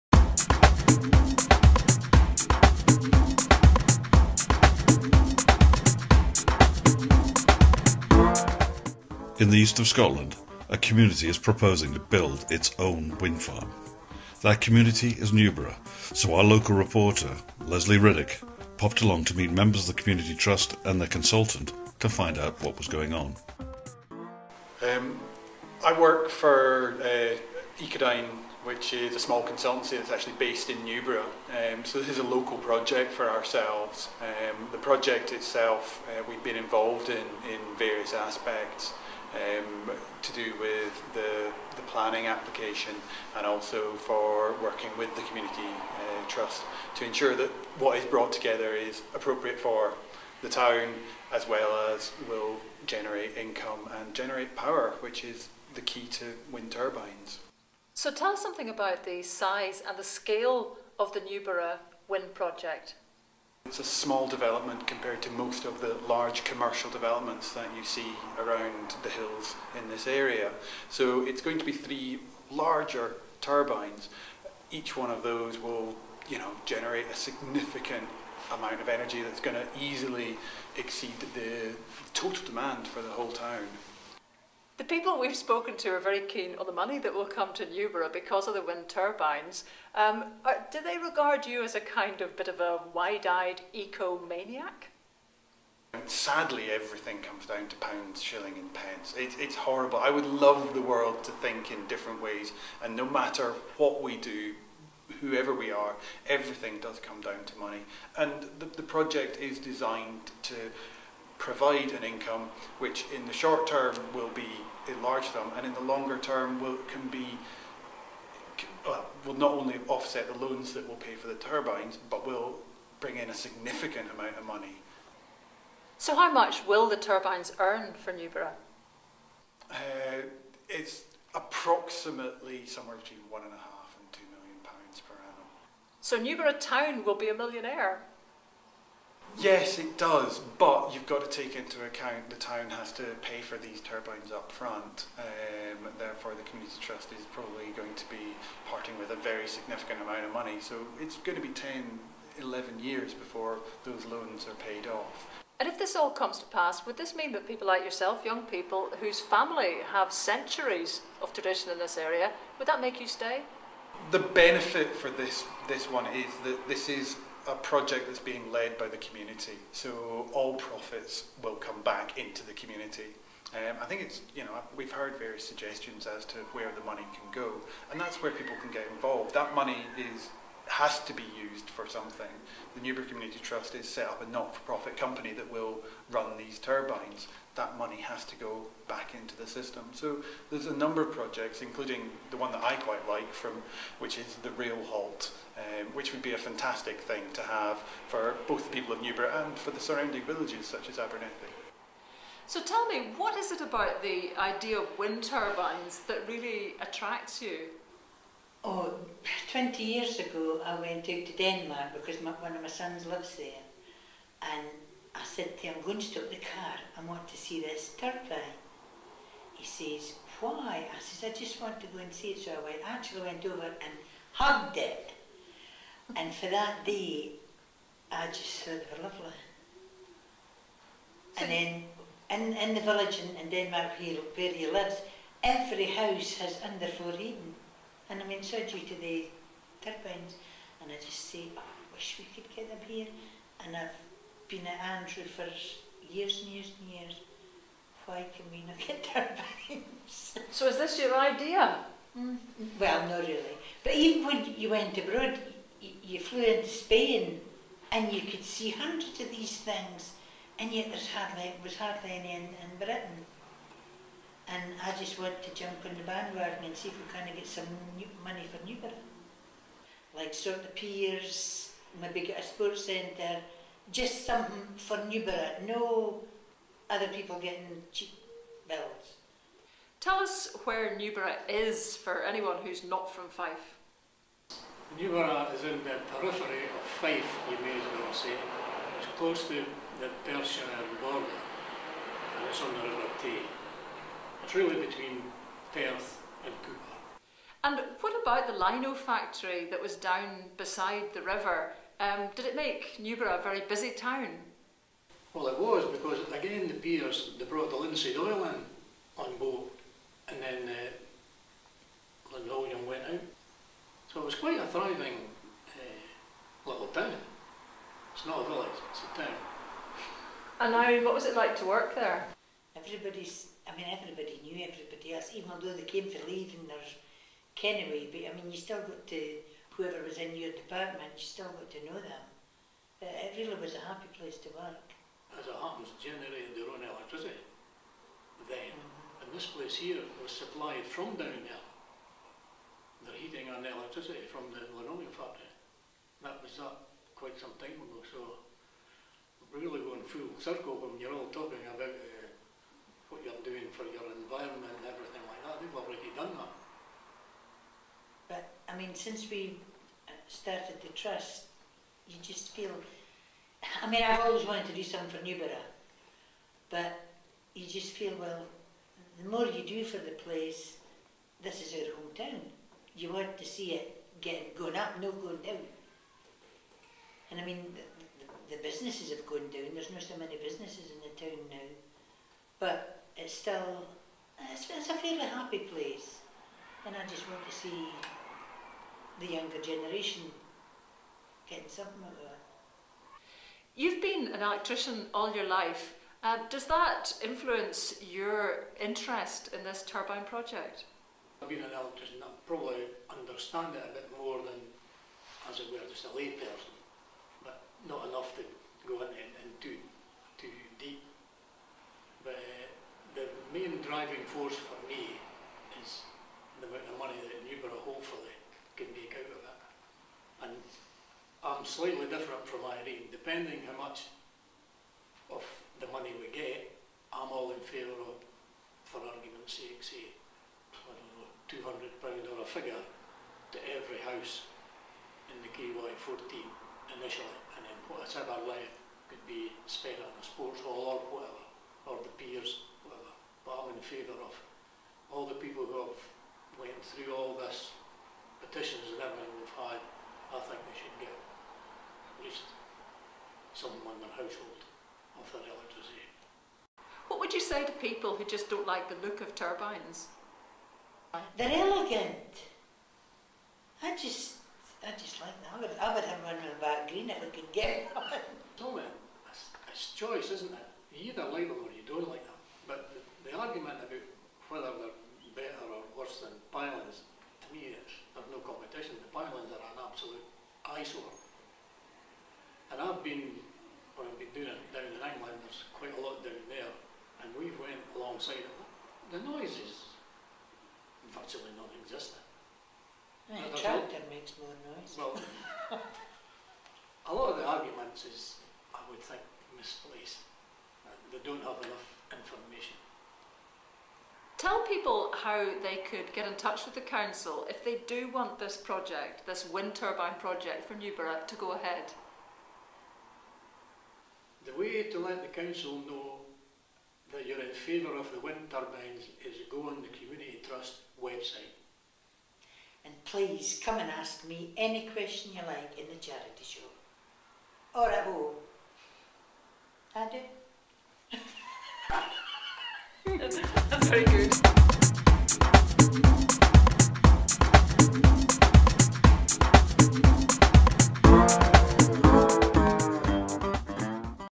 Newburgh Community Trust and the Wind Farm
This short video was shot with some of the local people involved in the Newburgh Community Trust Wind Farm proposal. Local broadcaster and journalist Lesley Riddoch met Keir Allen, Irene Cumming and Bob Batchelor. They explained some of the background, aspirations and details of the current proposal. This is an unique community led project in the east of Scotland. More information is available on the Trust's web site ;